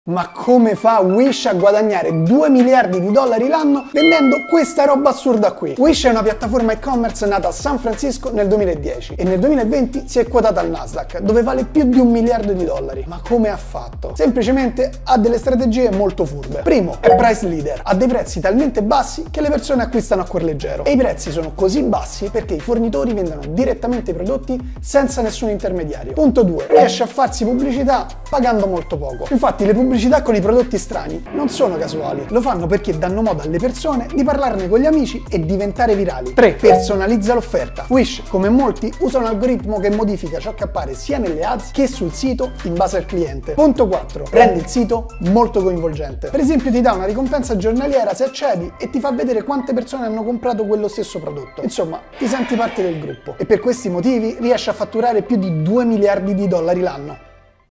Come Wish fattura miliardi vendendo cose inutili
Secondo Forbes, il fondatore di Wish è il miliardario più giovane del Canada.

Peter Szulczewski, classe 1981, è un businessman e ingegnere polacco. Da ragazzo si è trasferito insieme alla sua famiglia in Canada, dove ha ottenuto la cittadinanza.

Nel 2010 ha co-fondato l’e-commerce Wish insieme all’amico Danny Zhang. Stando ai calcoli di Forbes, il sito gli avrebbe fruttato un patrimonio di circa $1,4 miliardi.

Questo lo rende, sempre secondo Forbes, il miliardario più giovane del Canada.

Chi è il più giovane miliardario italiano?

Testi: Martina Ferraro
Montaggio: Maicol Morbidelli
Coordinamento: Edoardo Scirè